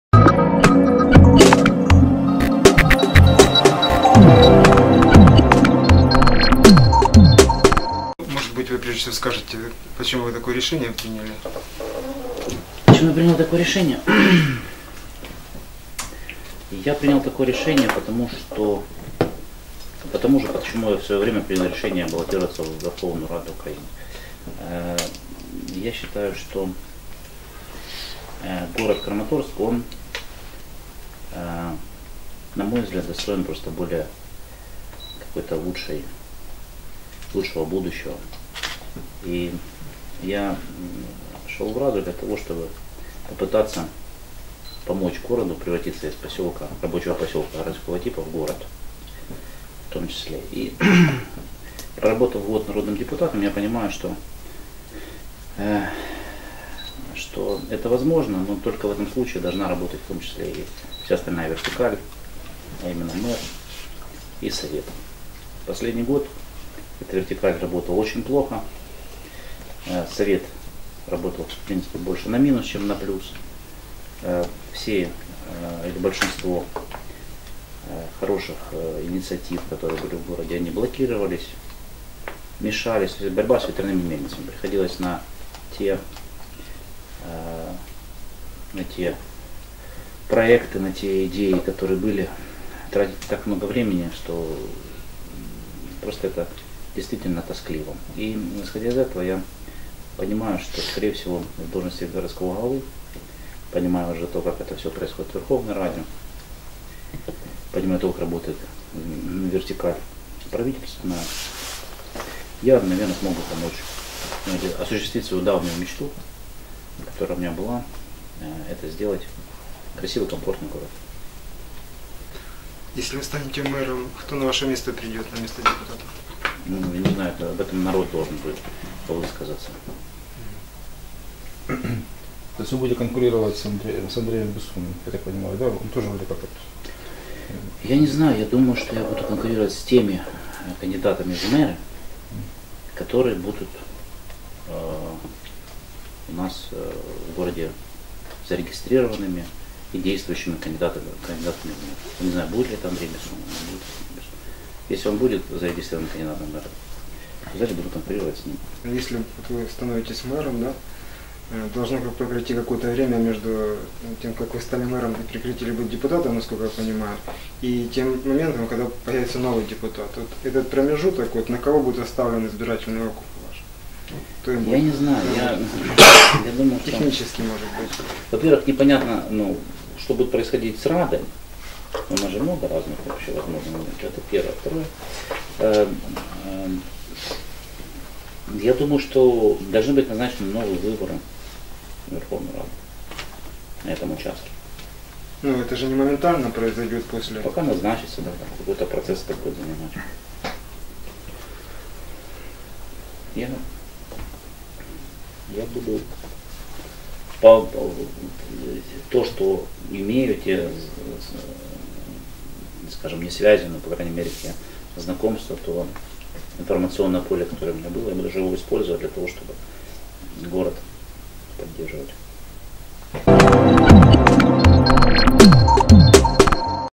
Нардеп из блока Порошенко собирается стать мэром Краматорска
Депутат Верховной Рады от Краматорска из фракции Блок Петра Порошенко Максим Ефимов выдвинул свою кундидатуру на пост городского головы

Депутат Верховної Ради від Краматорська з фракції блок Петра Порошенко Максим Єфімов висунув свою кундідатуру на пост міського голови